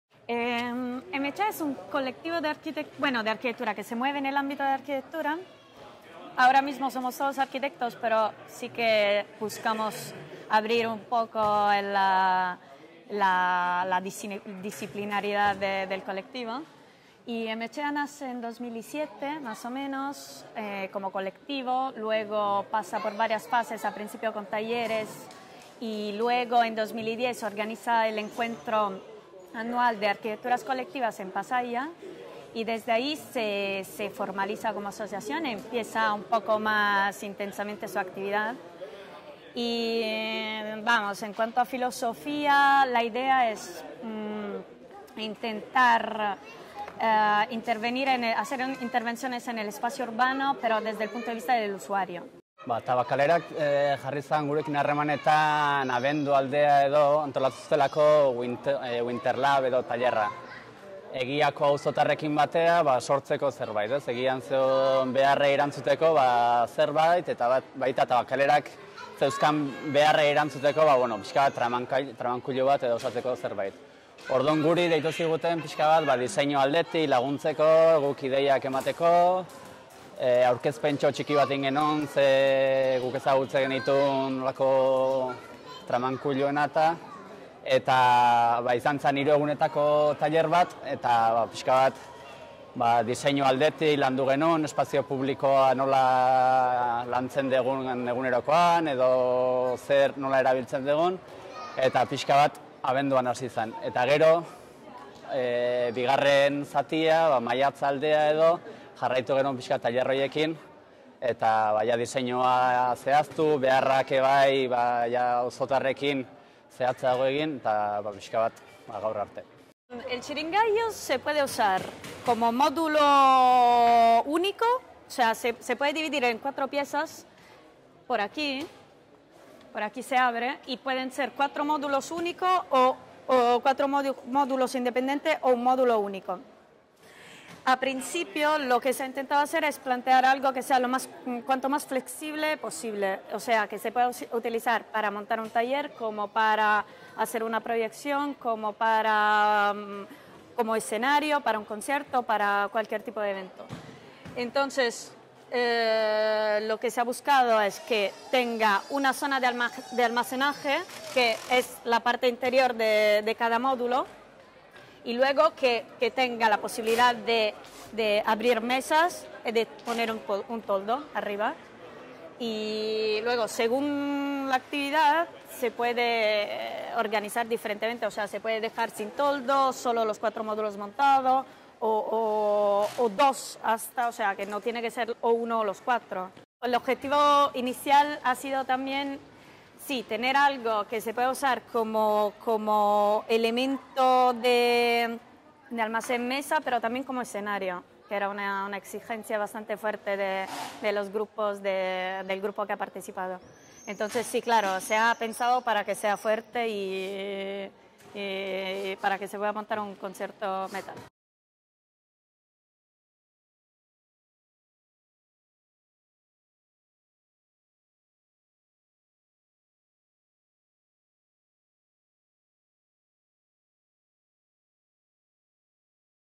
Manex Izagirre + Michela Lamedica. Txirin- gailua
Bideo honetan "Txirin-gailua" lantegiaren baitan Manex Izagirre eta Michela Lamedica, M-etxeako kideei egindako elkarrizketa ikus daiteke. (2014/07/3-5).
Entrevista realizada a Manex Izagirre y Michela Lamedica, miembros del colectivo M-etxea, mientras participaba en el taller "Txirin-gailua" (03-05/07/2014).